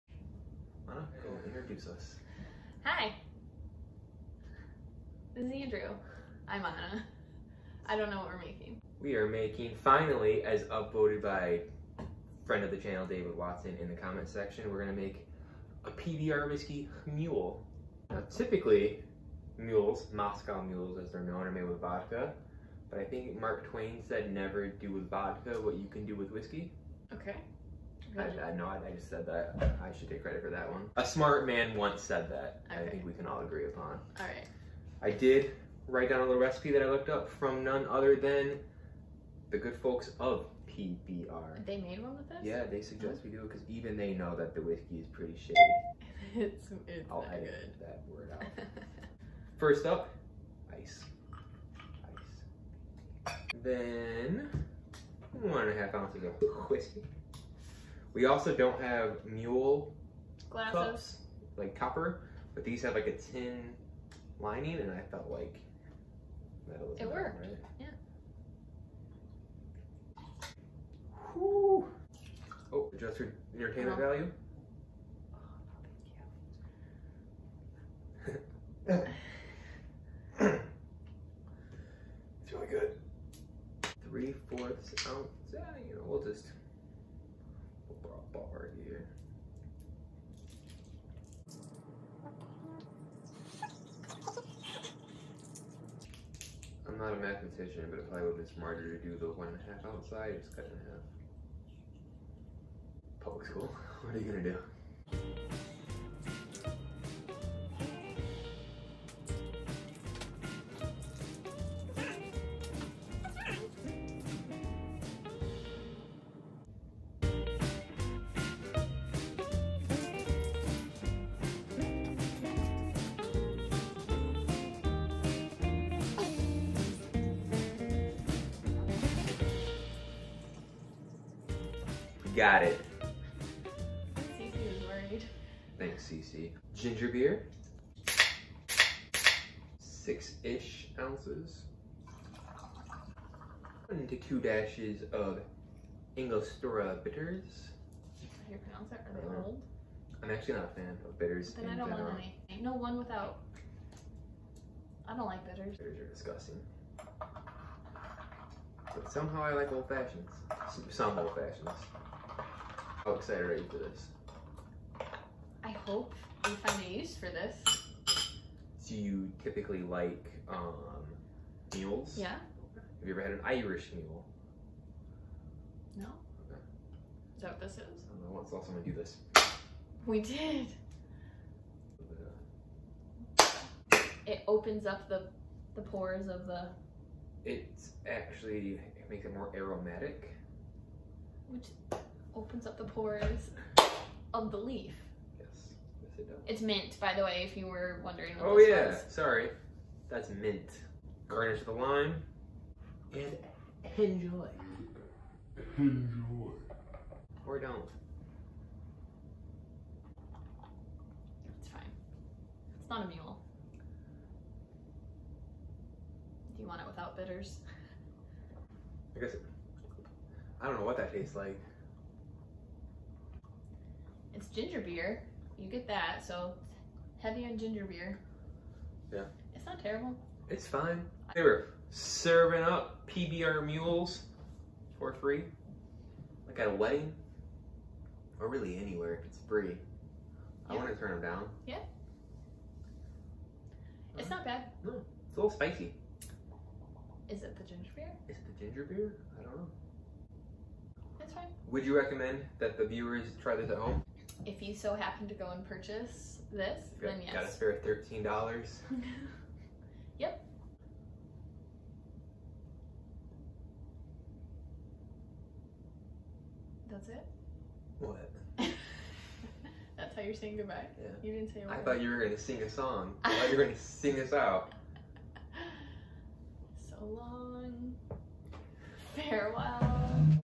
PBR Whiskey: the Redemption (Blue Ribbon Mule).
PBR Whiskey was terrible neat. How does it hold up in a cocktail? Great question! Let’s find out.